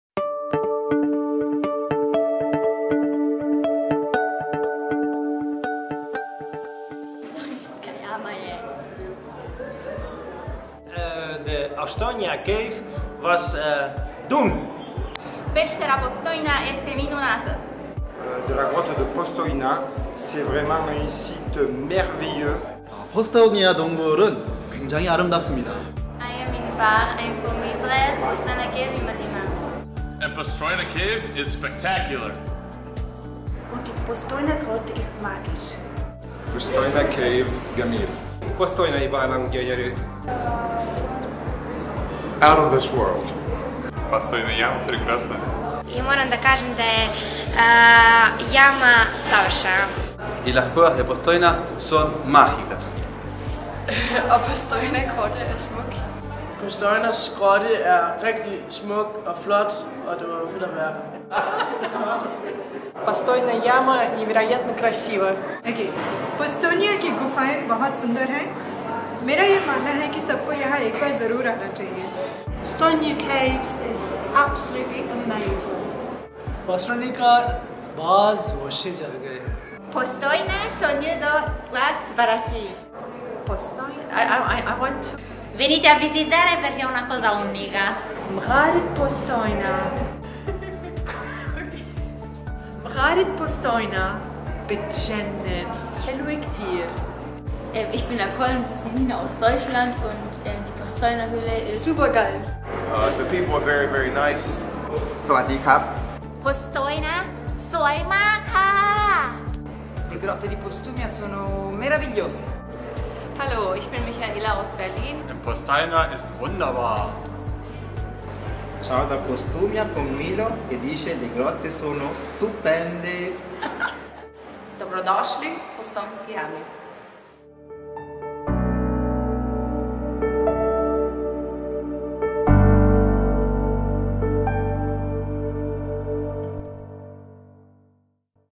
POSTOJNA CAVE - OUT OF THIS WORLD...
Postojna Cave (Postojnska jama) – impressions of visitors from all over the world after their visit to  the biggest show cave in Europe, Slovenia.
Unforgettable experience for everyone, regardless of age and continent.
Visitors have always been Postojna Cave's best ambassadors. It has been attracting visitors from all over the world for 200 years already.
Millions of visitors … countless impressions ….